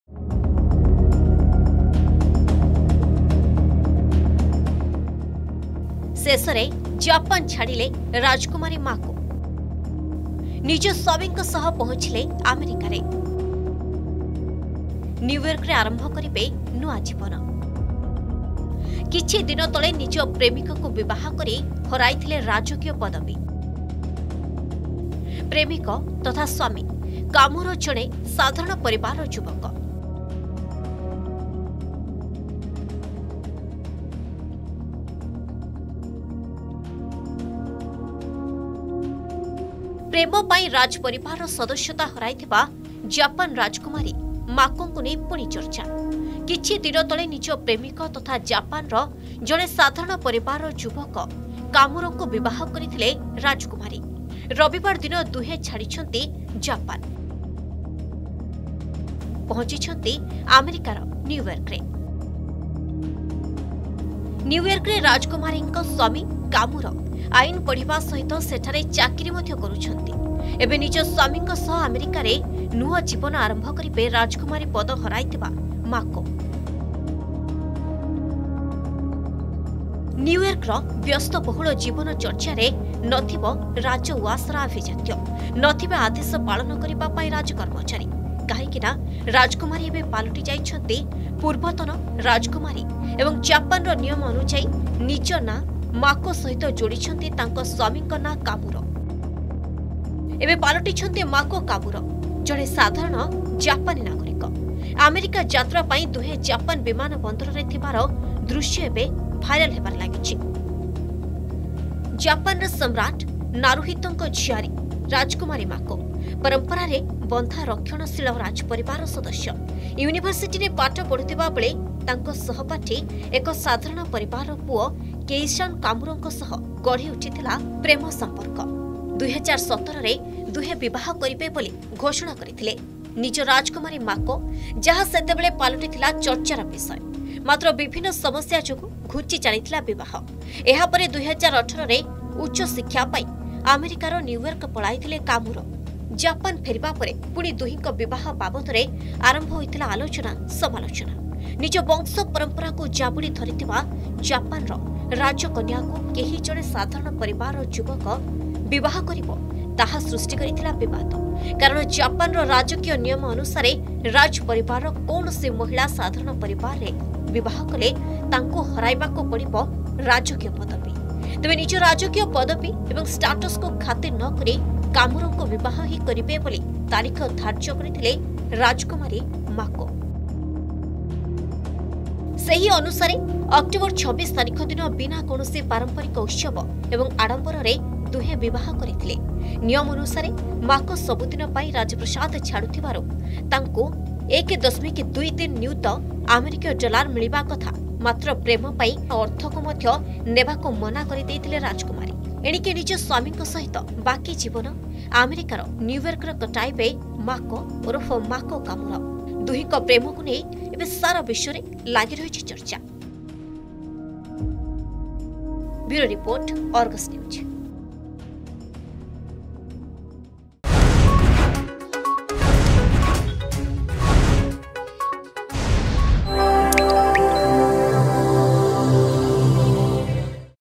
Japan's Former Princess Mako To Begin New Life With her Husband in United States
Japan's Former Princess Mako Gives up her royal title and moved to New York to start a new life with her husband Kei Komuro.